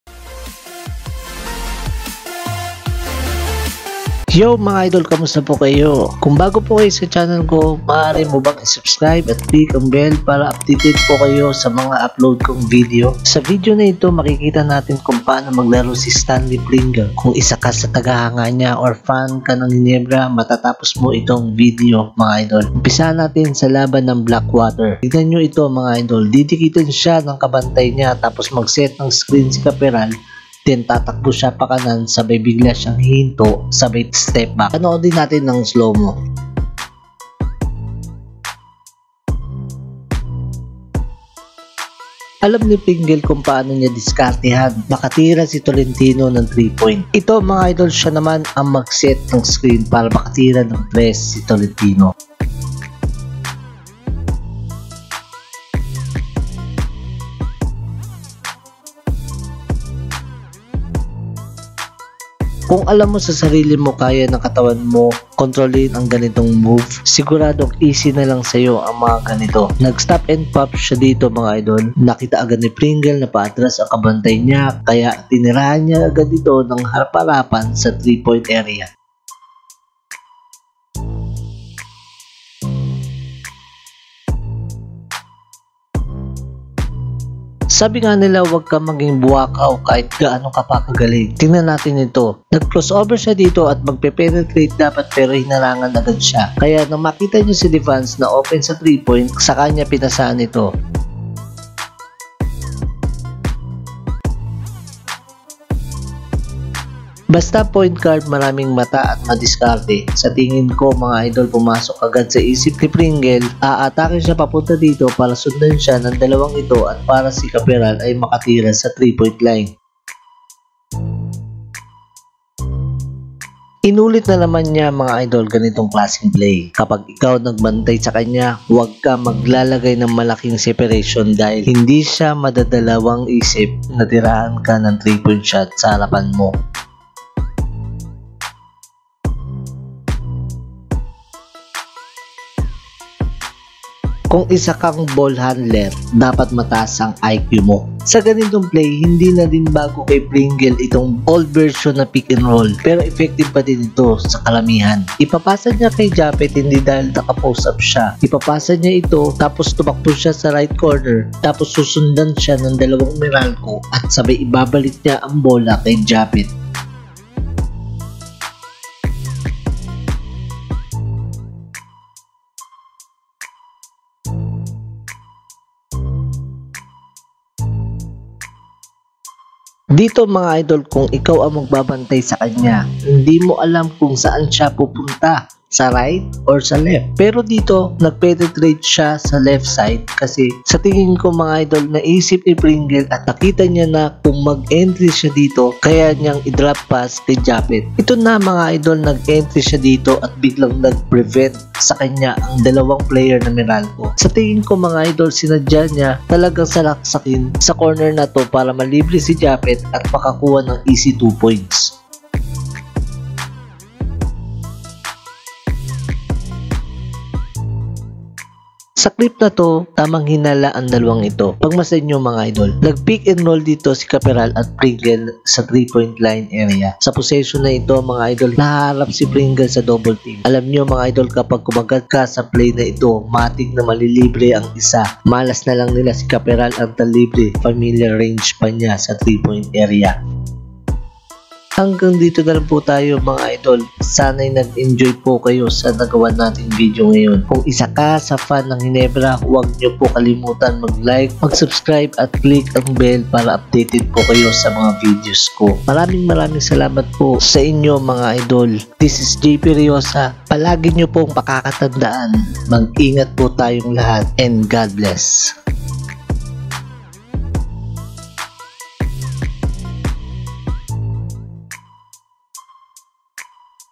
May secretong moves na hindi mahuhuli sino man ang galaw nya | hindi mapigilan si Stanley Pringle
*shout out
*topic
*Suggestion
*Opinion

This video is edited under by Fair use of Law of YouTube.